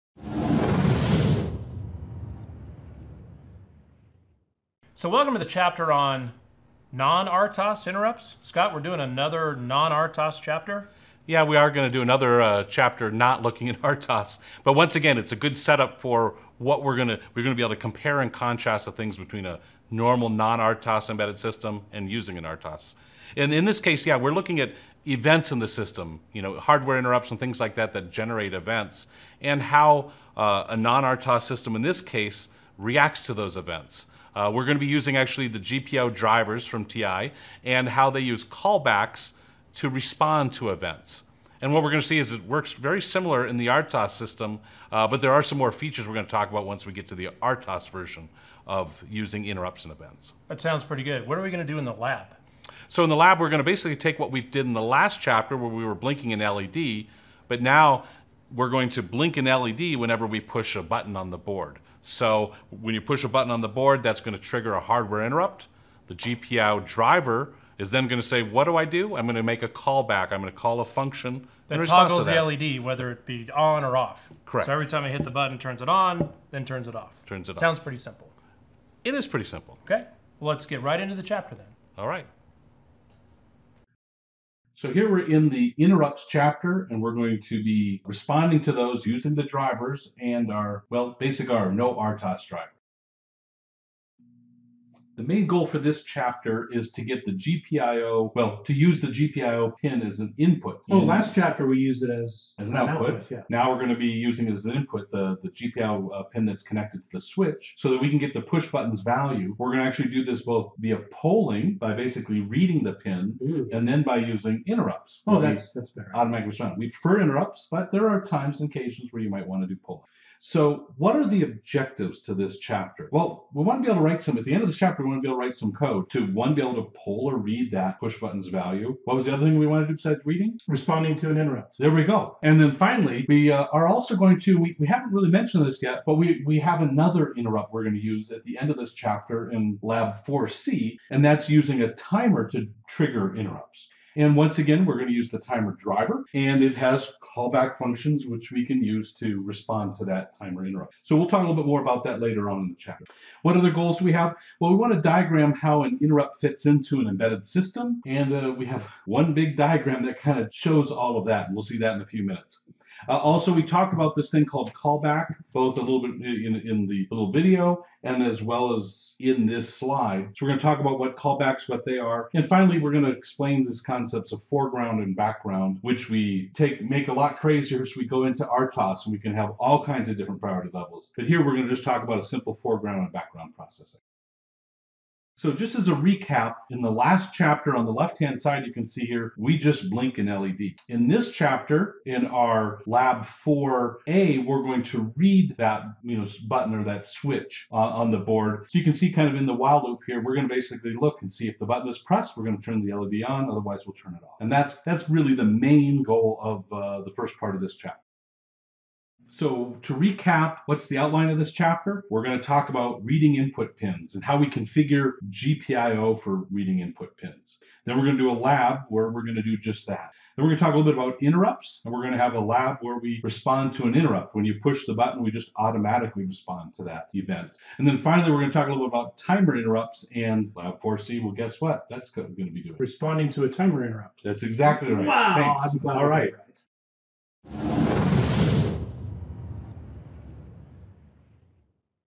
Getting Started with TI-RTOS: Chapter 4—drivers & interrupts
Learn more about the "Getting Started with TI-RTOS" workshop.

In this video, Eric Wilbur and Scott Specker of *Embedded Advantage [1]*
describe the objectives, agenda, and lab goals for Chapter 4 of
the *Getting Started with TI-RTOS [2] *workshop.

Chapter 4 covers these topics and answers these questions:

*Reading Input Pins*

 * How do I write code to read (poll) GPIO input from a push-button on the
   Launchpad?
 * How do I write code to configure GPIO to be input or output, set high or
   low and use pull-up resistors?

*Lab 4A – Reading GPIO input pin*

*Interrupts*

 * How do you write code to respond to a GPIO interrupt?
 * How do interrupts fit into the overall Embedded System Topology?
 * What is an interrupt and how do they work?
 * What is an event-driven system?

*Lab 4B – GPIO interrupt*

*Timer Interrupts*

 * How do you write code to respond to a timer interrupt using TI Drivers?
 * How are driver callback functions used in interrupt handlers?

*Lab 4C – Timer interrupt*